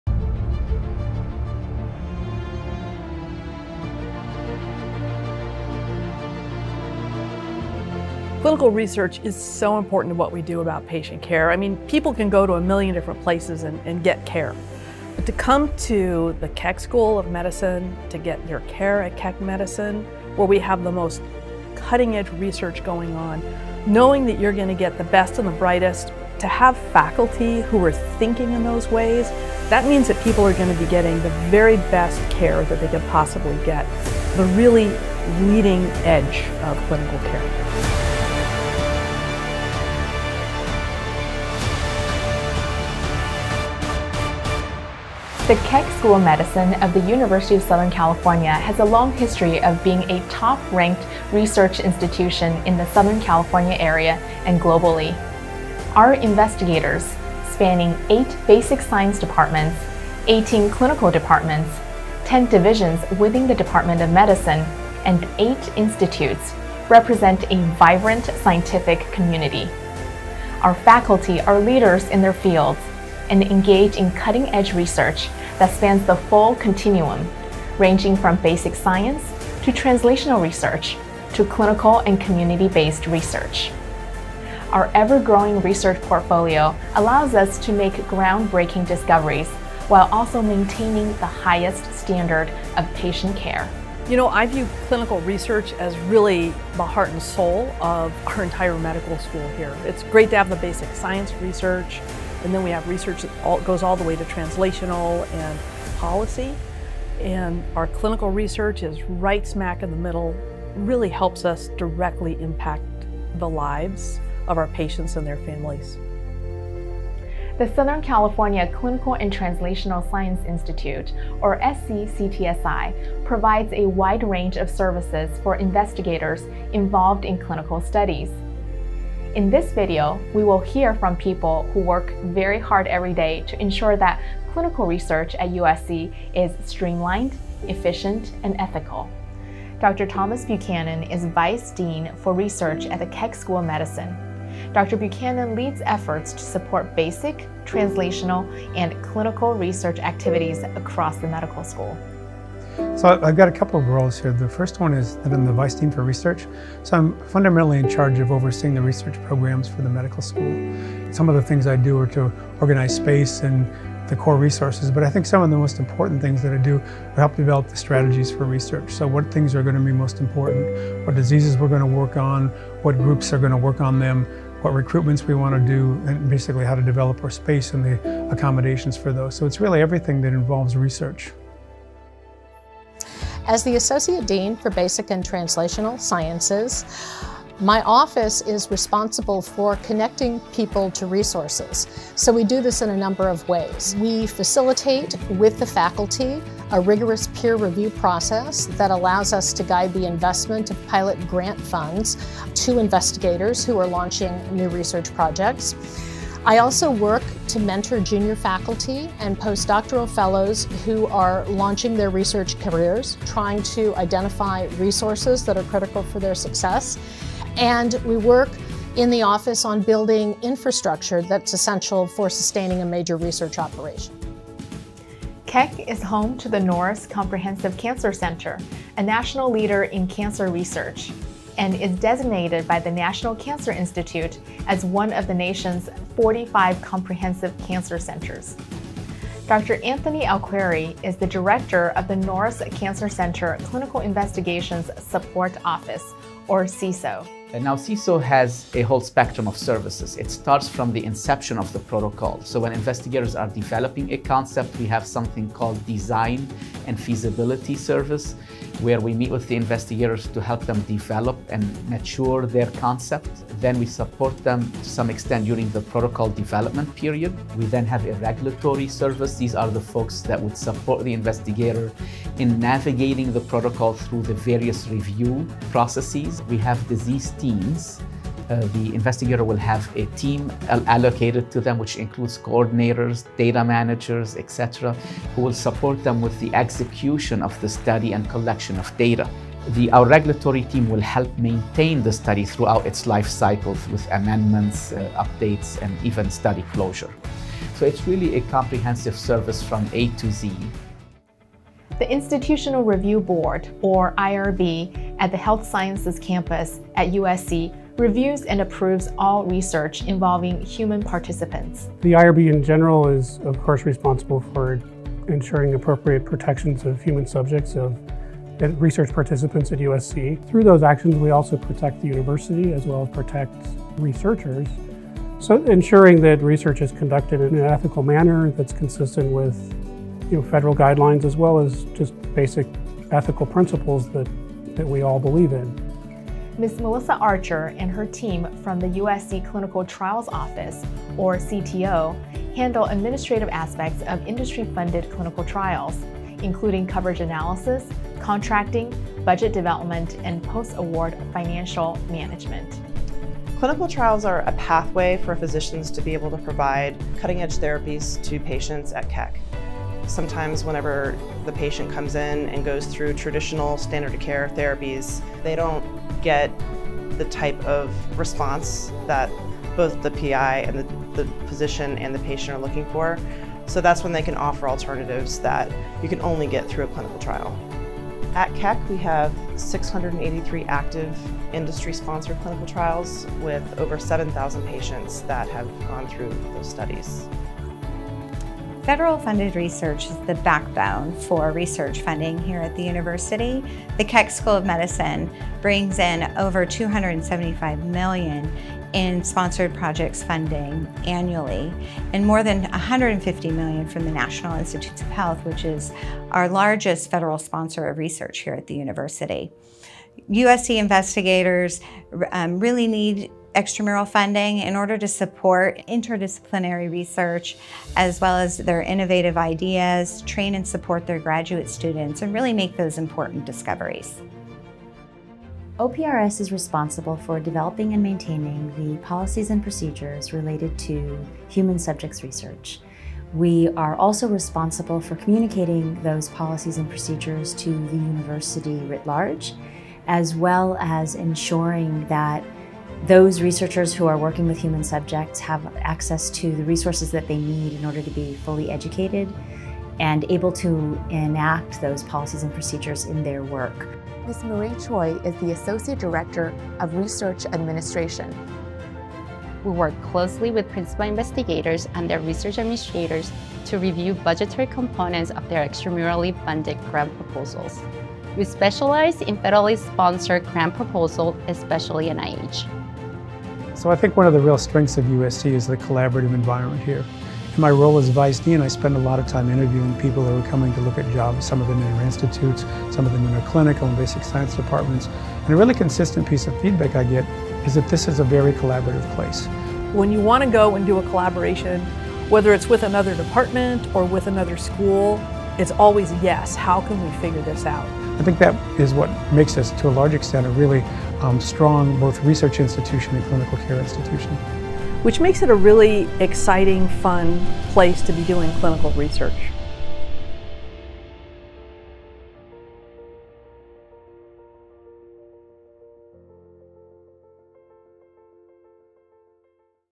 Clinical Research at Keck School of Medicine of USC
Check out our exciting new video on clinical research at the University of Southern California (USC). Leaders from various clinical research offices talk about how they help advance clinical research at USC.